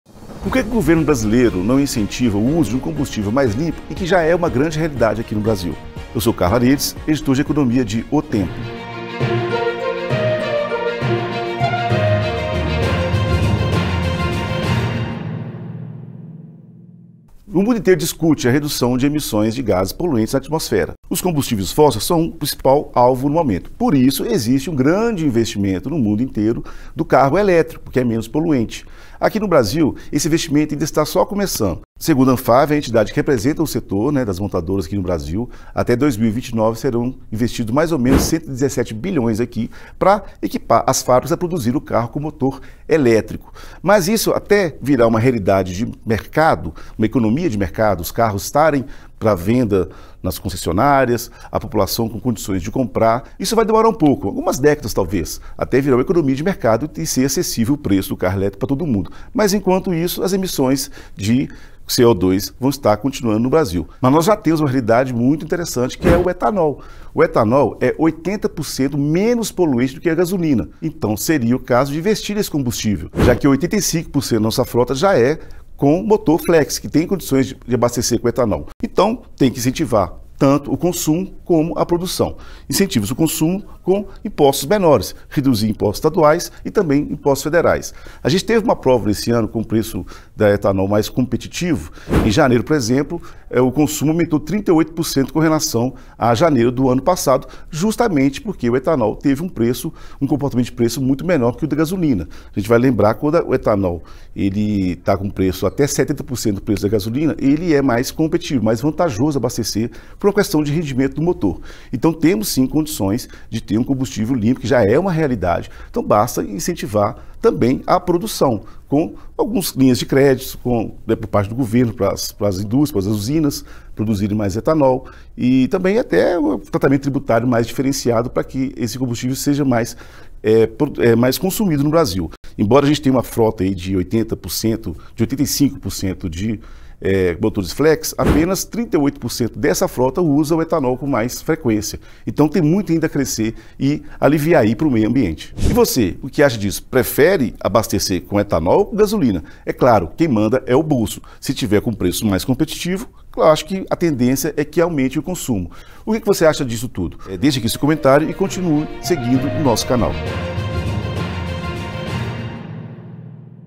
Etanol é 80% menos poluente que gasolina; como incentivar o consumo? | Análise Econômica
Enquanto vários países do mundo discutem e investem em energia limpa, no Brasil já há uma realidade que deveria ter mais atenção. O carro elétrico é a opção mais falada para substituir os combustíveis fósseis: derivados do petróleo, como a gasolina e diesel. Mas até ele virar produção em escala, com preços mais acessíveis, e dominar a frota brasileira, vai demorar talvez décadas. Enquanto isso, o etanol poderia ser uma opção mais usada no Brasil. O derivado da cana é 80% menos poluente do que a gasolina. Seria alternativa para 85% da frota, que tem a opção de ser abastecida com ele. Faltam mais incentivos para o consumo e produção, como, por exemplo, tratamento tributário diferenciado.

00:00 DEIXA SEU LIKE!
02:00 SE INSCREVA NO CANAL